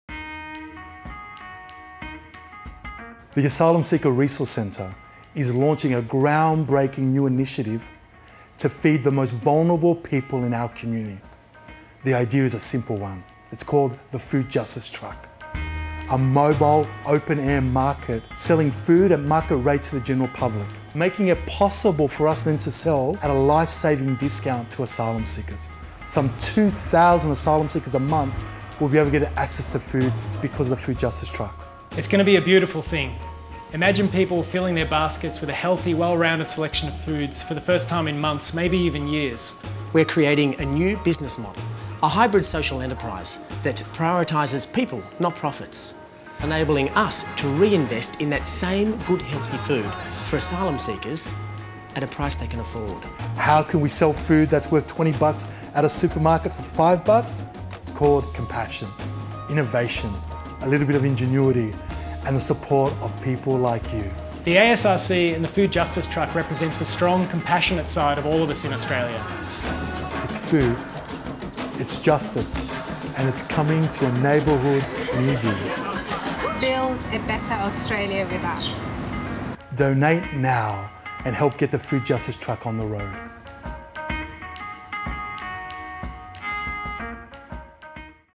ASRC Food Justice Truck - Crowd Funding Campaign
ASRC Food Justice Truck Crowd Funding Campaign - The FJT is a mobile food van going to multiple locations selling fresh food at market rates to the general public, while providing a 75% discount to asylum seekers. Most asylum seekers currently have no financial support and no right to work, the FJT will provide access to fresh food at a rate they can afford.